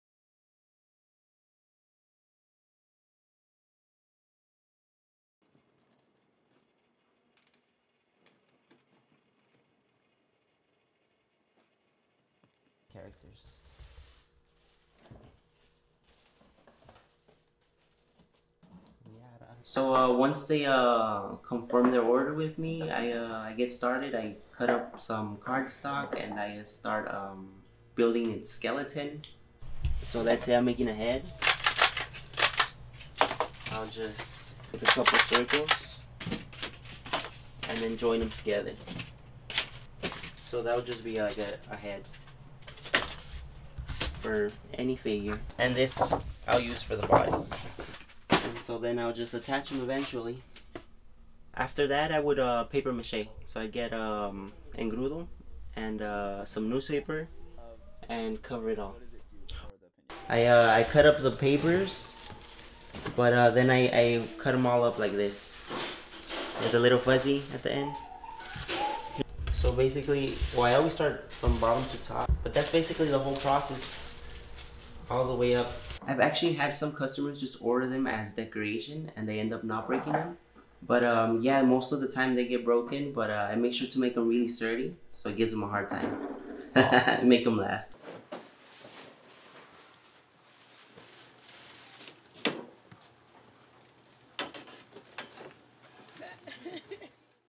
U.S. Postal Service unveils Piñata forever stamps
The U.S. Postal Service is rolling out its latest special edition postage stamps, paying homage to a Hispanic tradition that has evolved over centuries to become a universal symbol of celebration. (AP video by Ty O'Neil and Eugene Garcia)